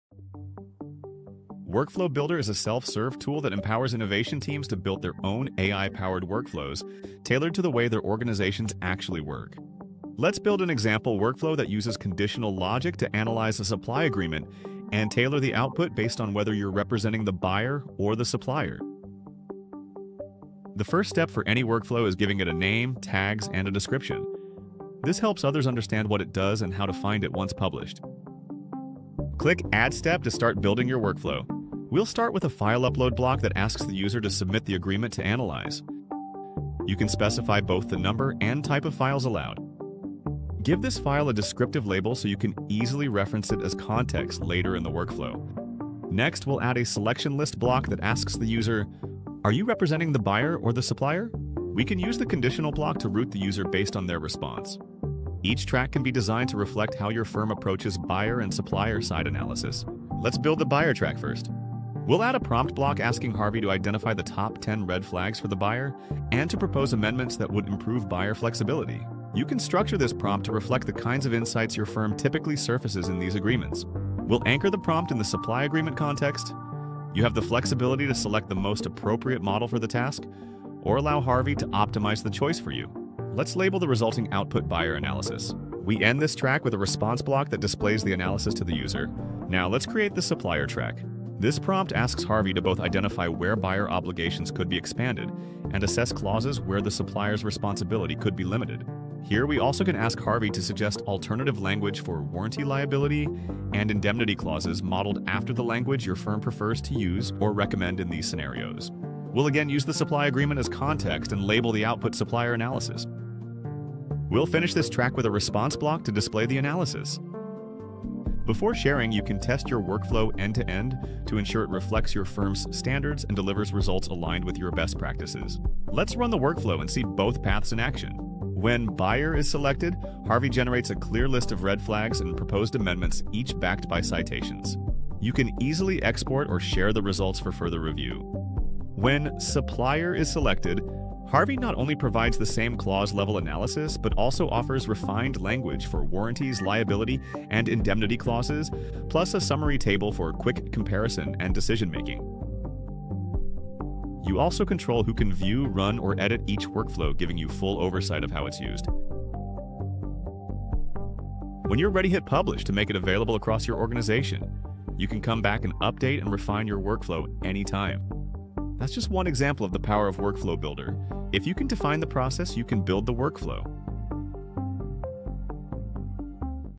Build Your Own Legal AI Agents with Harvey Workflow Builder
Introducing Workflow Builder, now available to all Harvey customers.

With Workflow Builder, legal teams can design and deploy custom AI workflows that capture their firm’s unique expertise, tone, and processes. Transform proprietary knowledge into reusable systems that deliver high-quality legal work, every time.

Built on Harvey’s domain-specific AI, Workflow Builder empowers Innovation and Knowledge teams to turn best practices into one-click, guided experiences. Whether you’re triaging complex documents, generating end-to-end workflows, or scaling your firm’s differentiators, Workflow Builder makes it possible — no coding required.